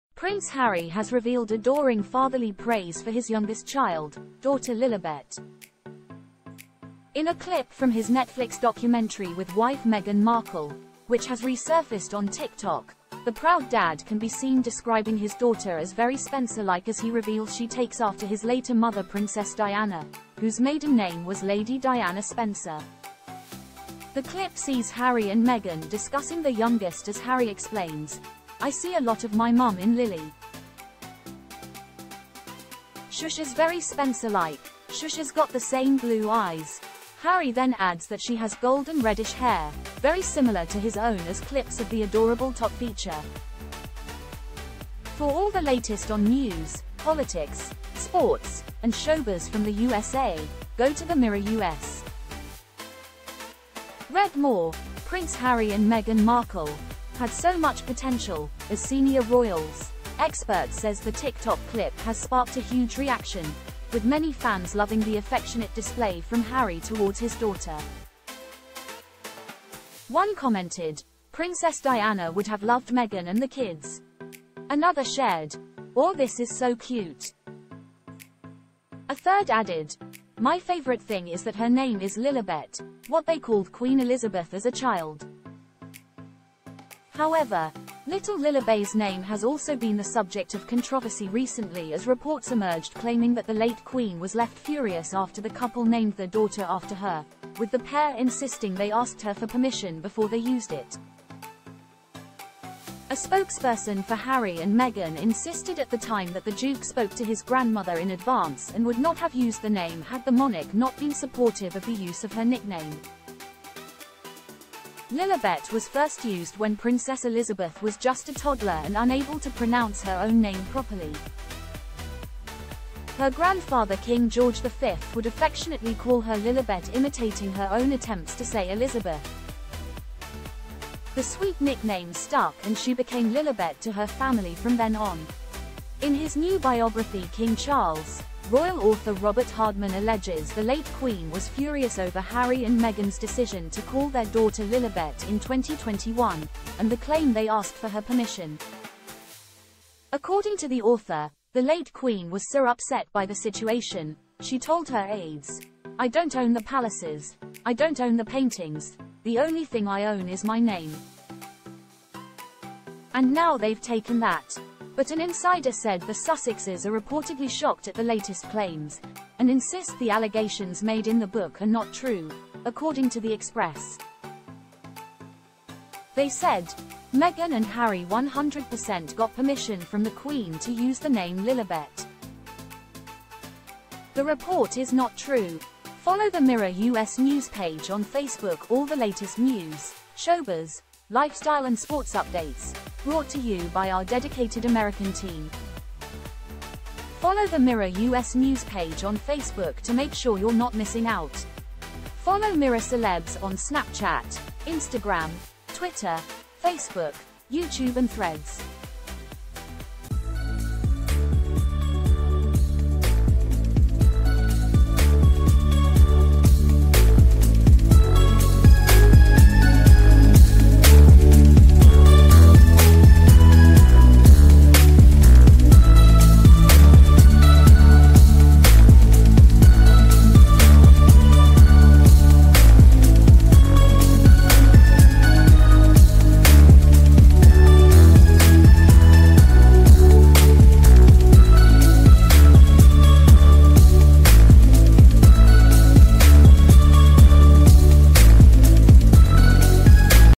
Prince Harry's sweet praise for Lilibet resurfaces amid royal baby name row
Prince Harry has revealed adoring fatherly praise for his youngest child, daughter Lilibet.  In a clip from his Netflix documentary with wife Meghan Markle, which has resurfaced on TikTok, the proud dad can be seen describing his daughter as "very Spencer-like" as he reveals she takes after his later mother Princess Diana, who's maiden name was Lady Diana Spencer.  The clip sees harry and Meghan discussing their youngest as Harry explains: "I see a lot of my mum in Lili. She's very Spencer-like...